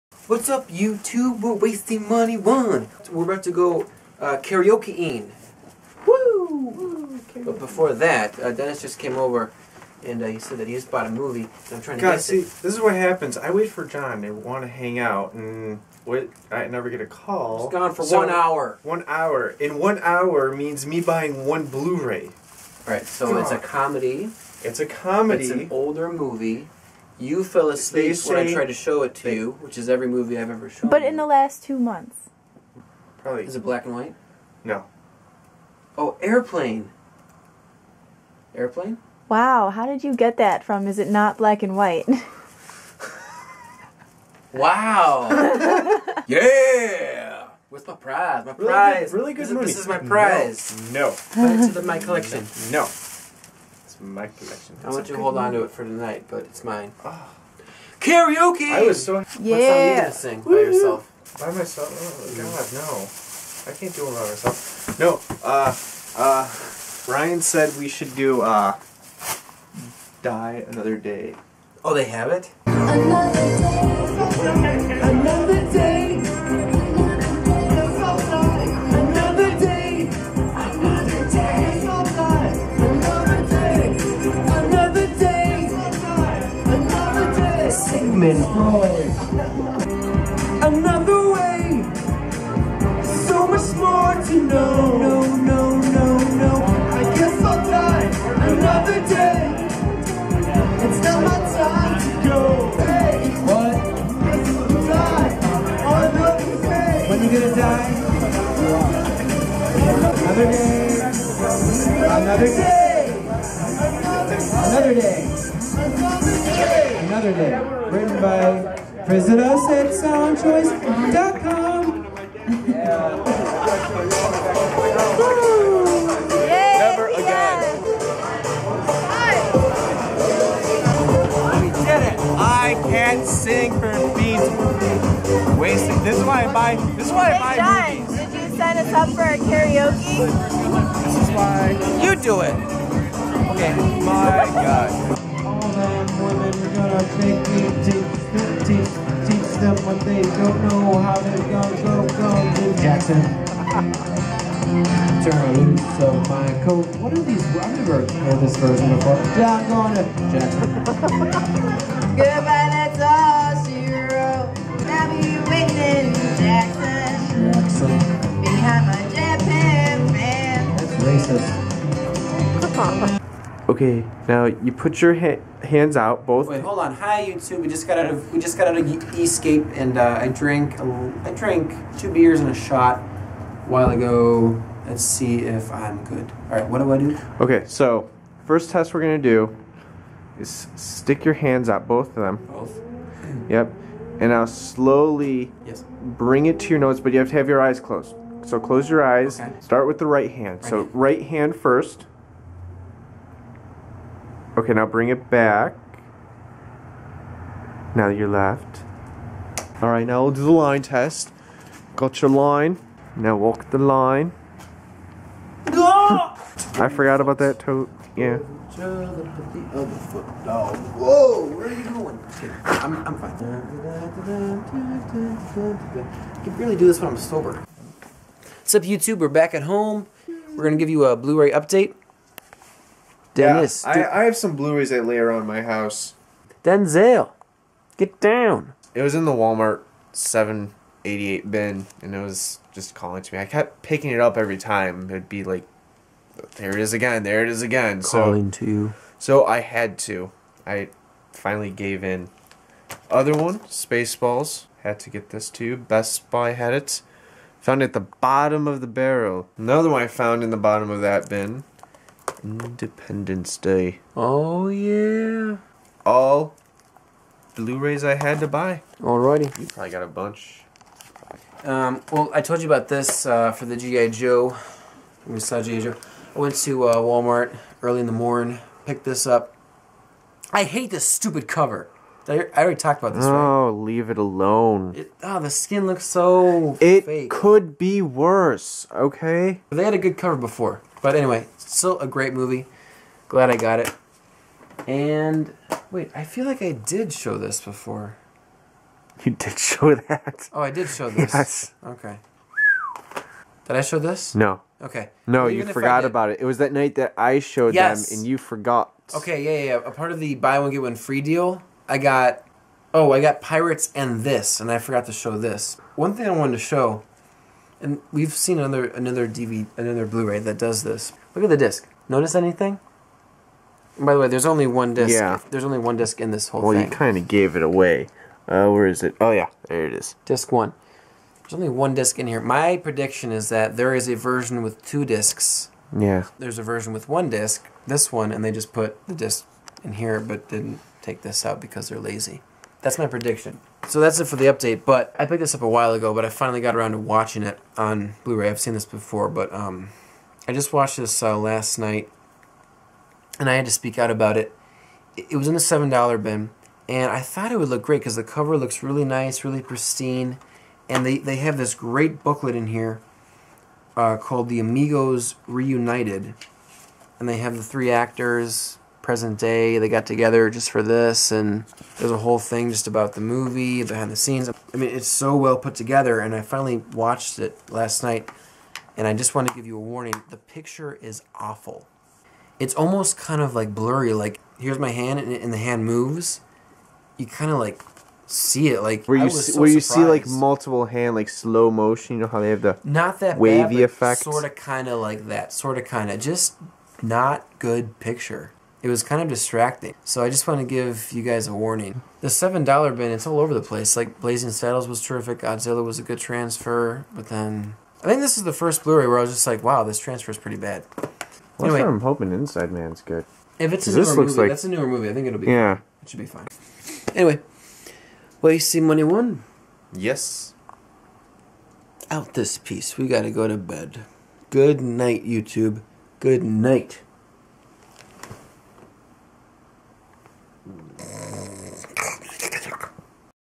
Karaoke Night! (& Blu-ray Update)
Lo and behold the blu-rays bought by John and Dennis!  But before all the movie goodness, it's KARAOKE NIGHT BABAY!!!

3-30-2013